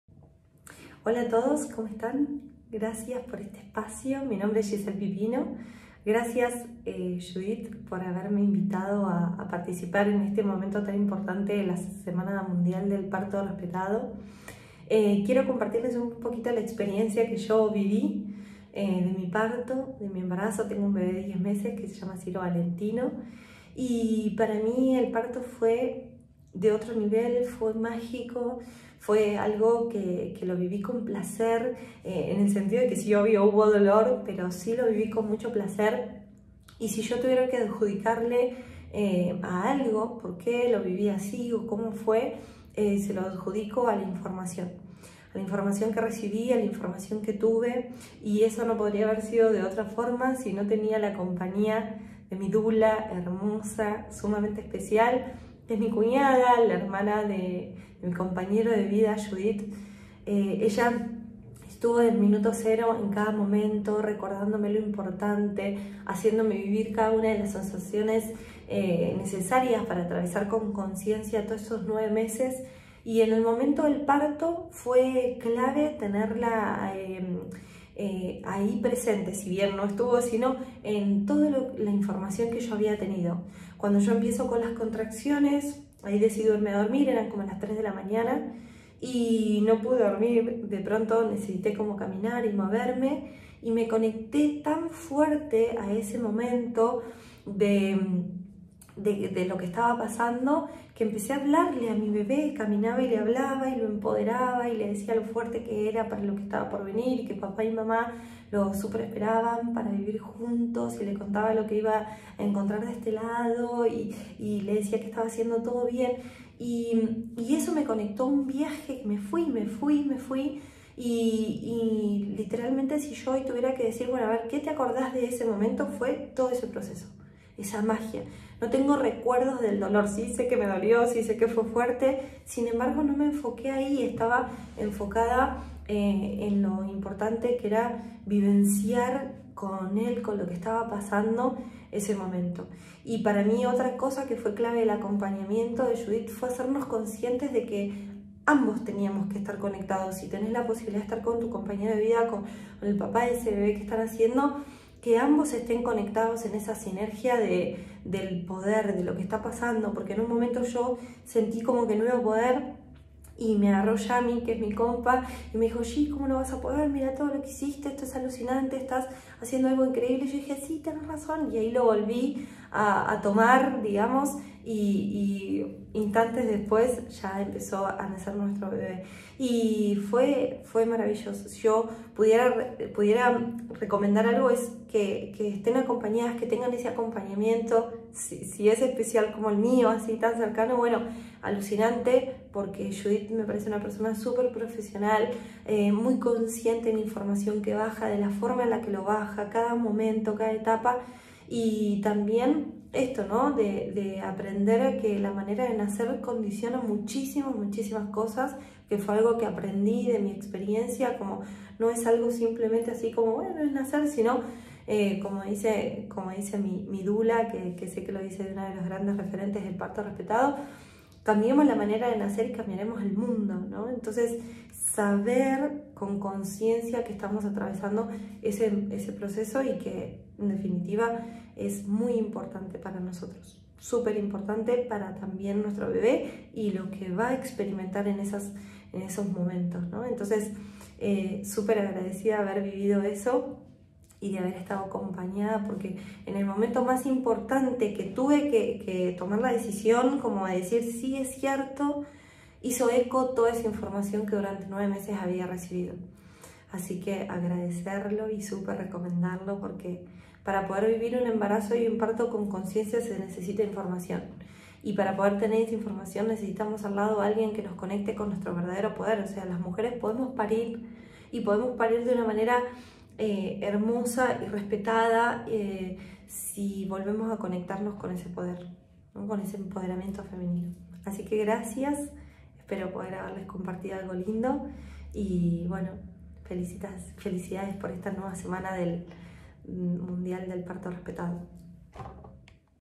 TESTIMONIO DE ACOMPAÑAMIENTO
Culminamos la Semana Mundial del Parto Respetado (SMPR) con el testimonio de una mamá que comparte su experiencia de embarazo y parto.

Te invitamos a compartir el programa para seguir aportando "Luz y conciencia a lo que somos y a lo que hacemos con lo que somos".